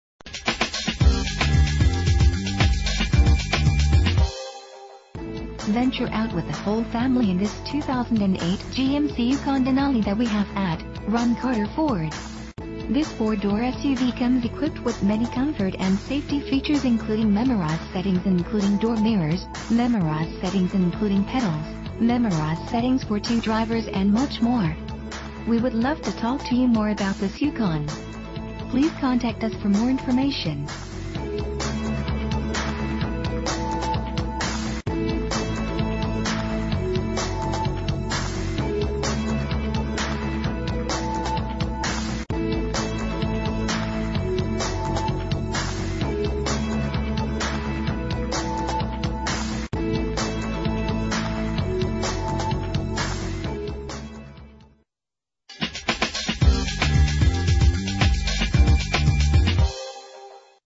2008 GMC Yukon - Alvin TX
Year: 2008
Make: GMC
Model: Yukon
Trim: Denali
Engine: 6.2 liter 8 cylinder 16 valve
Transmission: 6-Speed Automatic
Color: Tan
Mileage: 93130
Address:
3005 FM 528